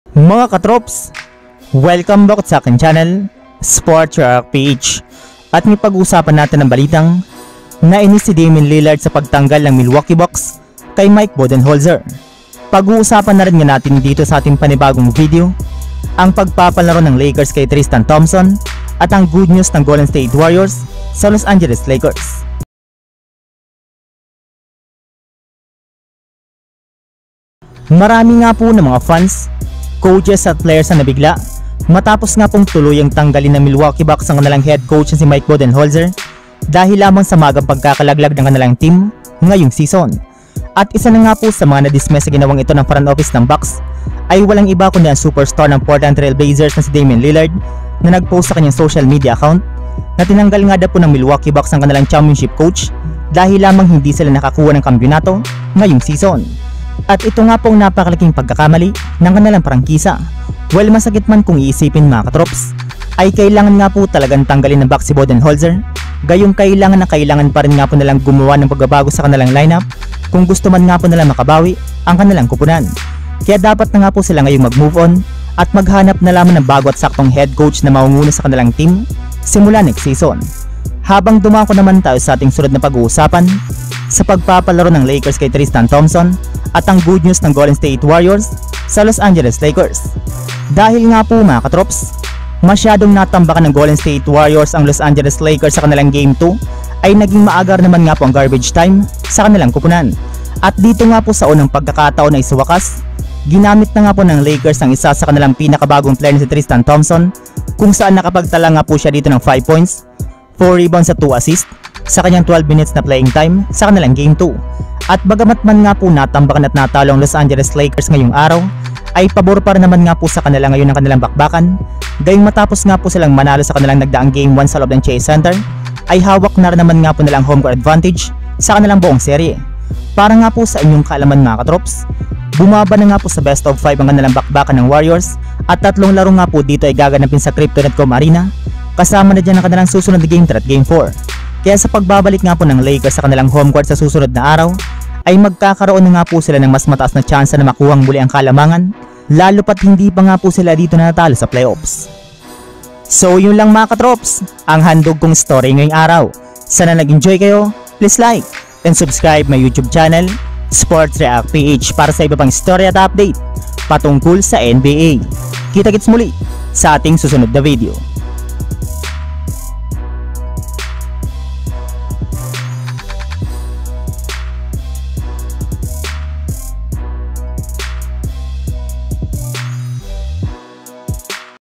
JUST IN: WARRIORS MAY "GOOD NEWS" sa LAKERS na IKATUTUWA ng FANS | LILLARD GALIT sa BUCKS
Ang pag uusapan nga natin ngayon ay tungkol sa Los Angeles Lakers at Golden State Warriors.